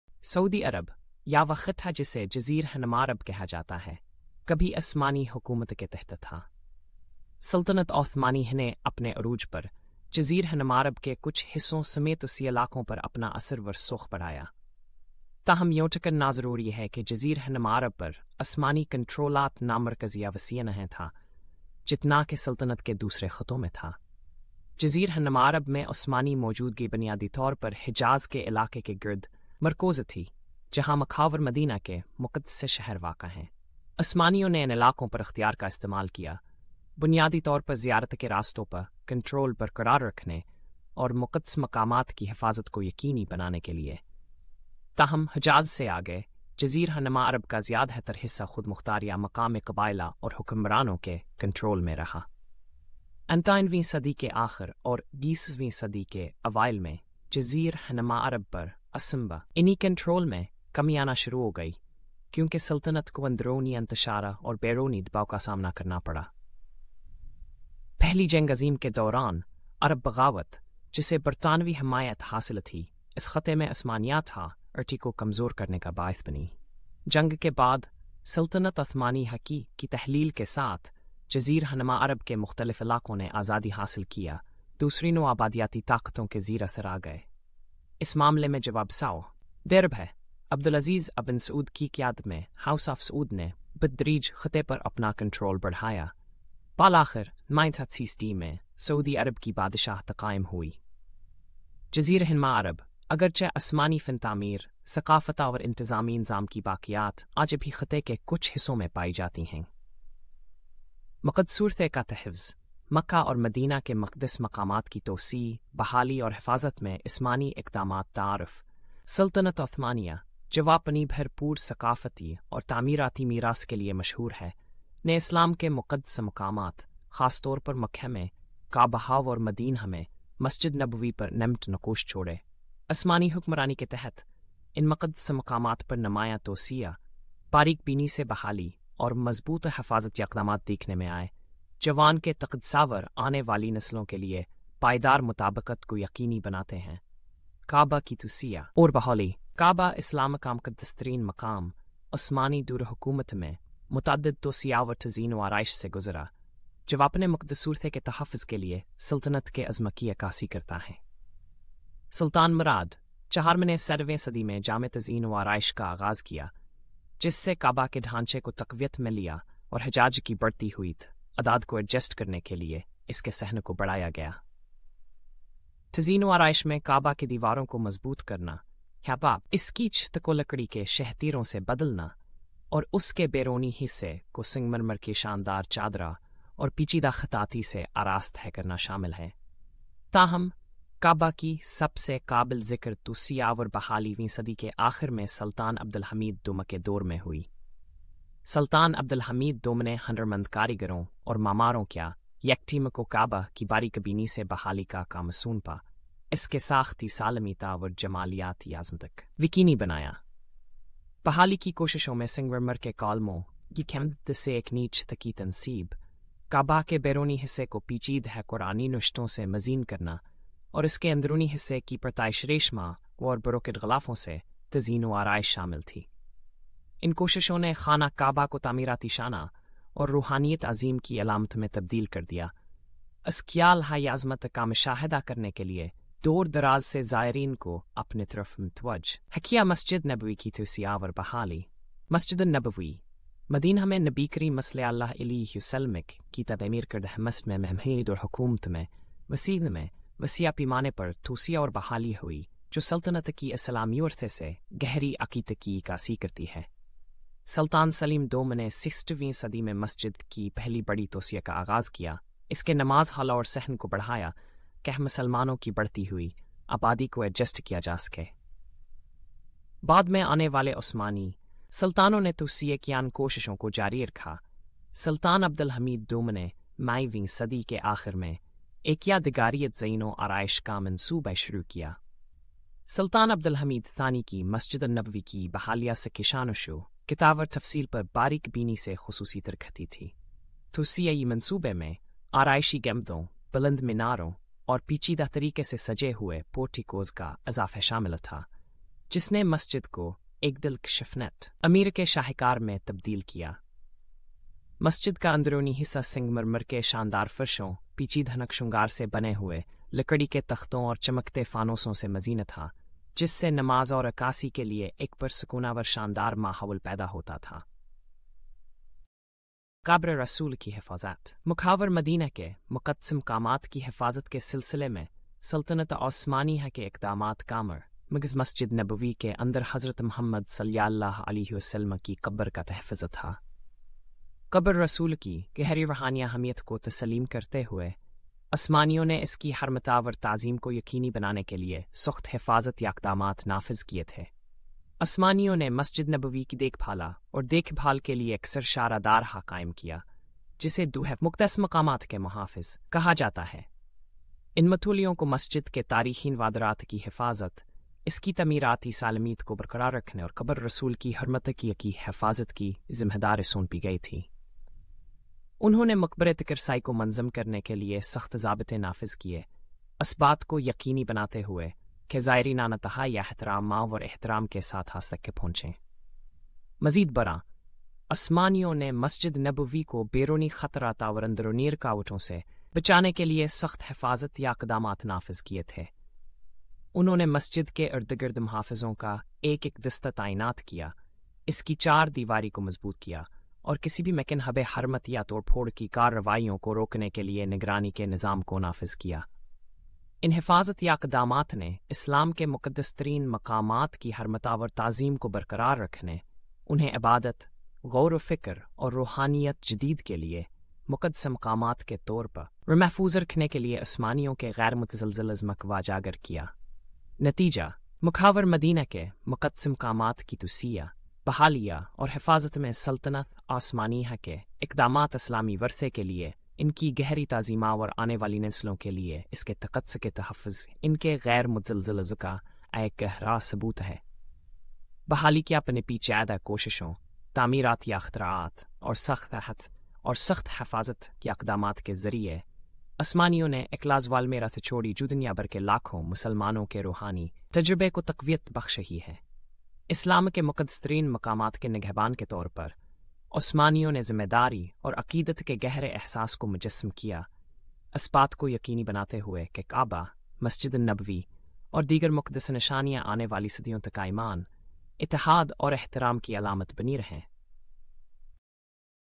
عقیدہ کے محافظ: اسلامی مقدس مقامات کی حفاظت اور ان میں اضافہ کرنے میں عثمانی میراث
اسلام کے مقدس ترین مقامات کے تحفظ، توسیع اور تحفظ کے لیے سلطنت عثمانیہ کی گہرائیوں کا جائزہ لیں، بشمول مکہ میں خانہ کعبہ اور مسجد نبوی۔ مدینہ۔ ان کی تعمیراتی اختراعات، بحالی کی کوششوں، اور سخت حفاظتی اقدامات کا جائزہ لیں، اسلامی ورثے کے لیے ان کی گہری تعظیم اور آنے والی نسلوں کے لیے مقدس مقامات کی حفاظت کے عزم کو اجاگر کریں